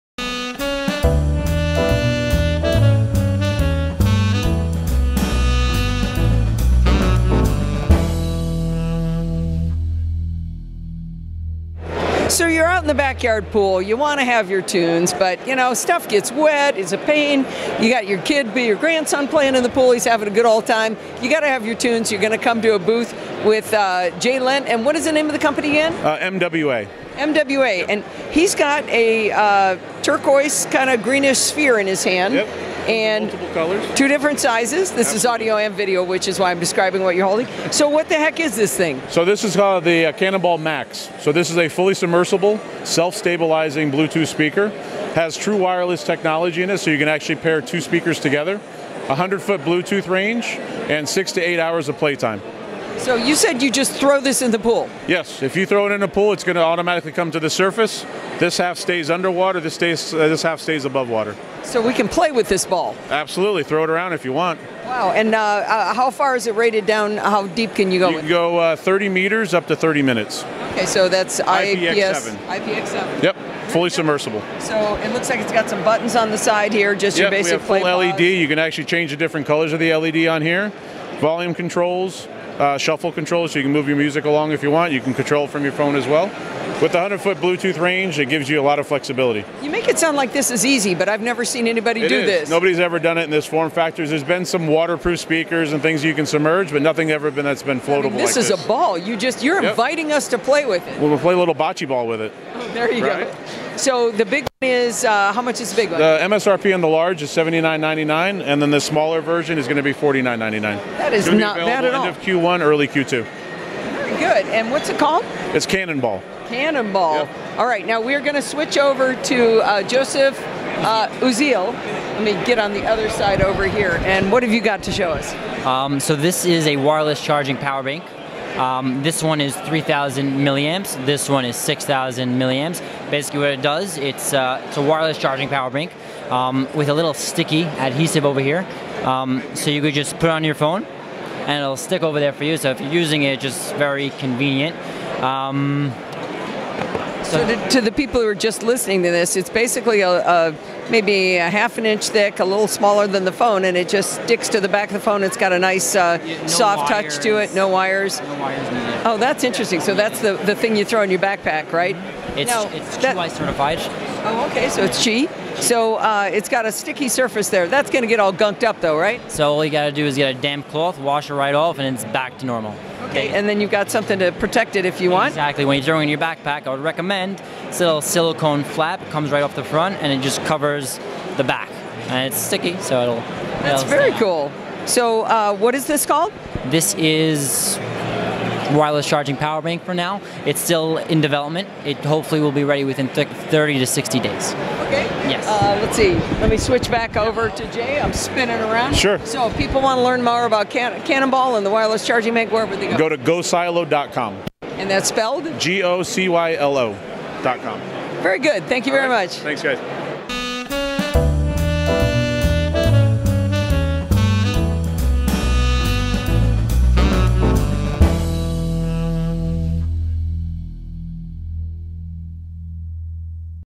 CES 2019: Cylo Cannonball Speaker and Qi Charging Bank
The first, described by Jay, is the Cylo Cannonball, a floating waterproof speaker that can be thrown and caught, submerged or placed on a solid surface.  It’s equipped with a rubberized shock-absorbing cover and flat surface buttons. The rugged Cannonball is rated IPX7 so it’s built to withstand the elements including water, dirt, sand, snow and dust. The Cannonball can pair with Bluetooth-enabled devices and it supports wireless calls with a built-in mic and ranges up to 100 ft. The second product, described by Joey, is two wireless charging power banks for mobile phones that use Qi charging, including later iPhone models. The first provides 3,000 mAh of charge and the second provides 6,000 mAh. Both power banks are designed to stick to the back of your mobile phone for charging but can be removed and reapplied as many times as you desire. These devices are in development and should be available in the second quarter of 2019. The setting is the Showstoppers show floor at the Wynn in Las Vegas.